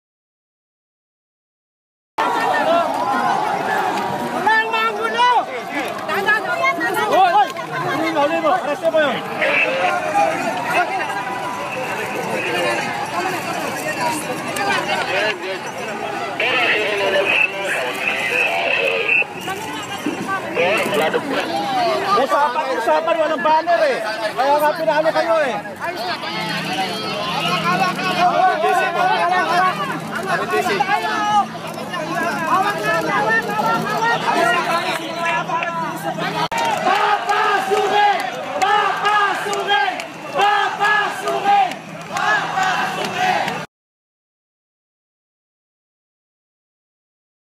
Protesters vs police at EDSA Tayo
More videos on Rappler Follow Rappler for the latest news in the Philippines and across the globe. Protesters and police clash at the EDSA Tayo prayer vigil at Edsa Shrine after one police officer grabbed one protester's poster Follow Rappler for the latest news in the Philippines and around the world.